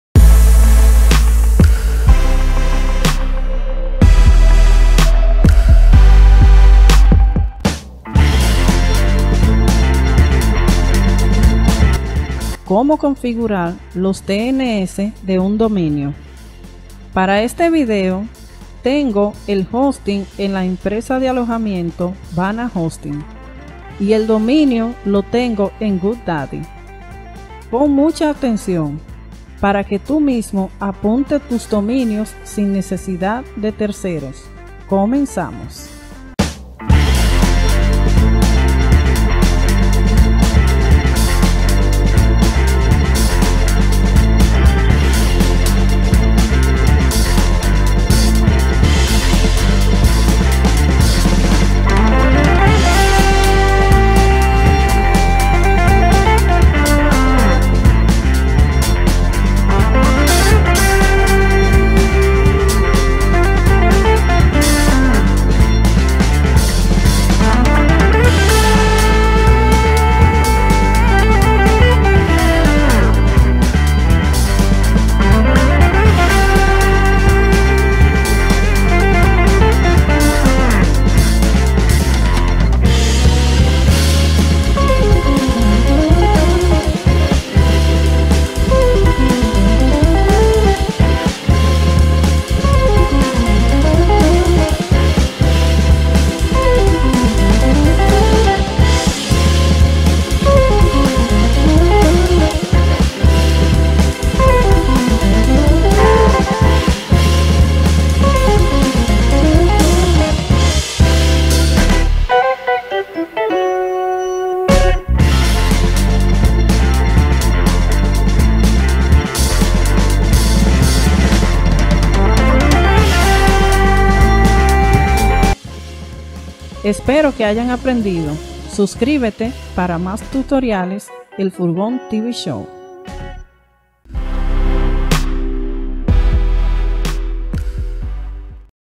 ¿Cómo configurar los DNS de un dominio?
cuantas veces tiene que pagarle dinero a un web master para que te haga una tarea tan simple como apuntar tus DNS a tu dominio. aqui te enseñamos.

Bienvenidos a "ElFurgonTVShow" donde podrás disfrutar de los Análisis y debates de interés general. noticias dominicana, chisme del espectaculo y mas..